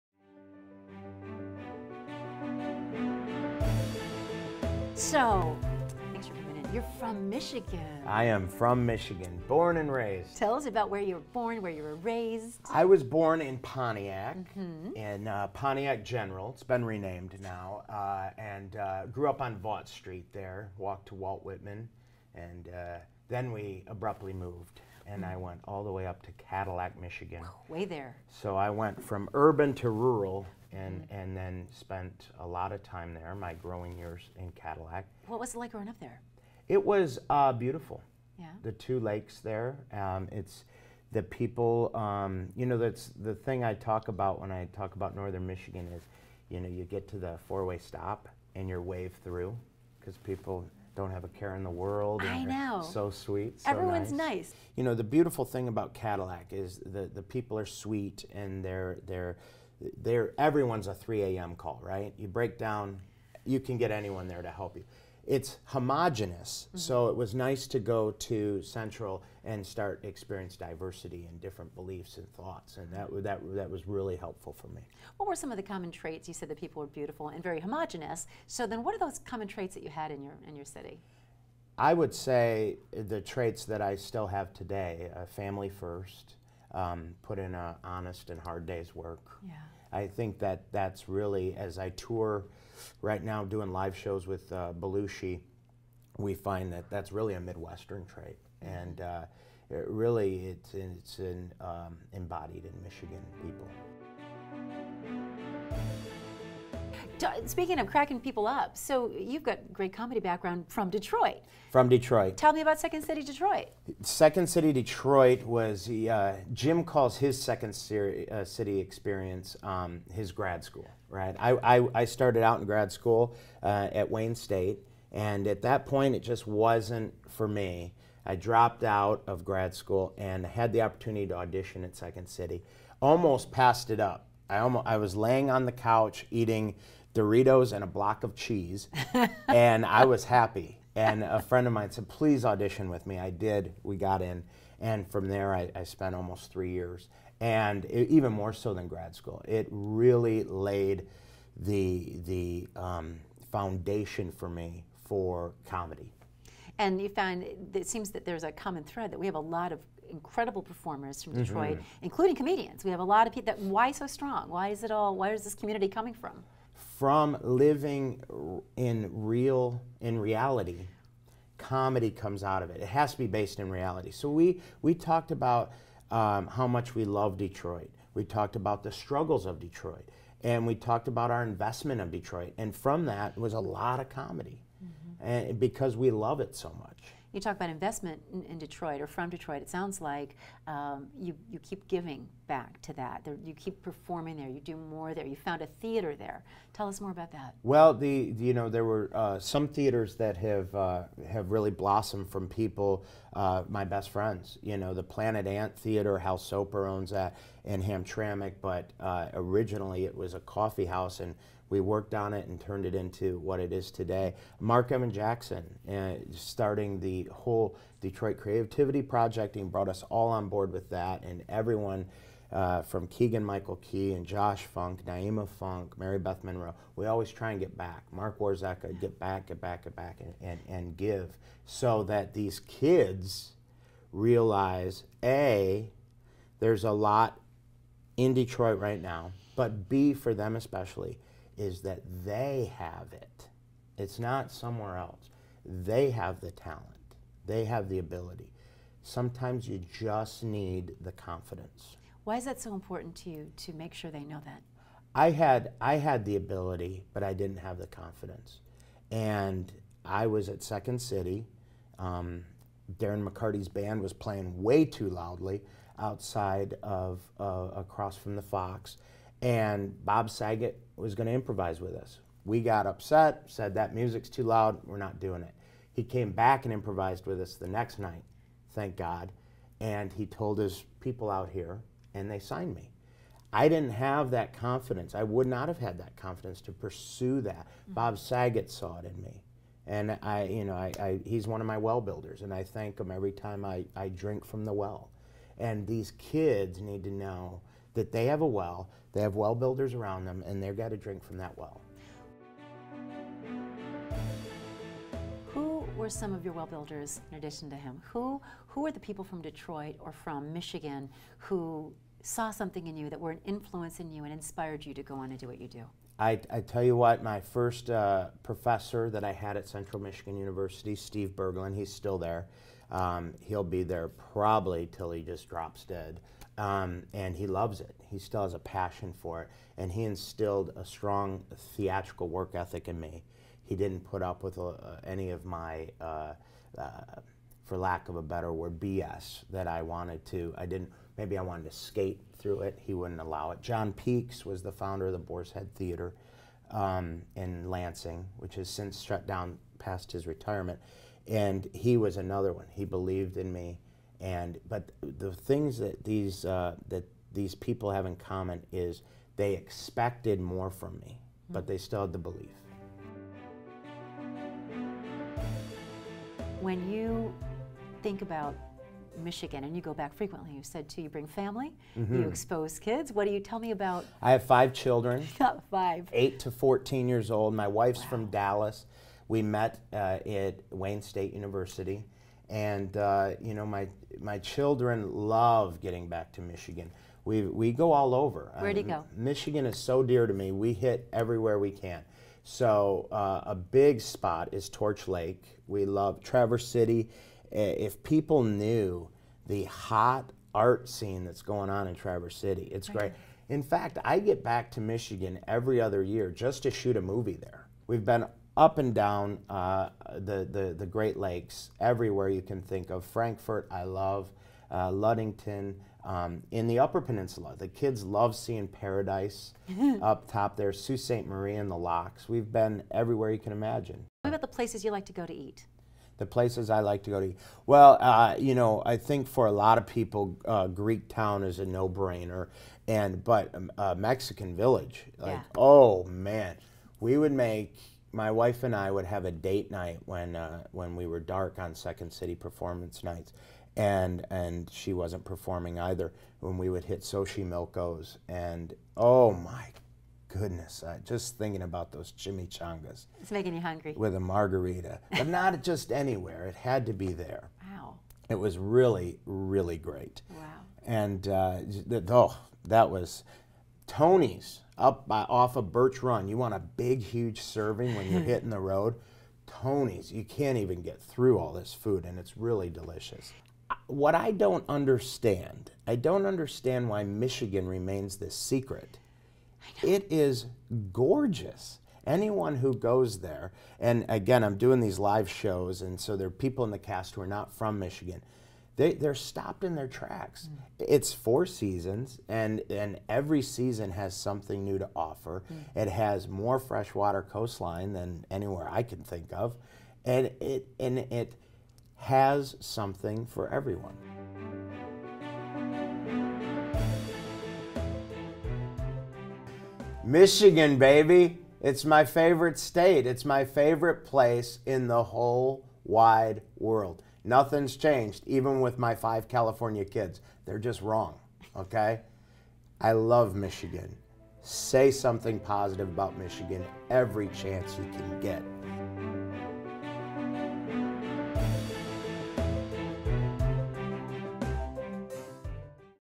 Actor Larry Joe Campbell Describes His Love for Michigan
Funny man, Larry Joe Campbell, sits down with host, Suzanne Sena, to discuss his Michigan roots, career development, and places he still loves to visit throughout the state.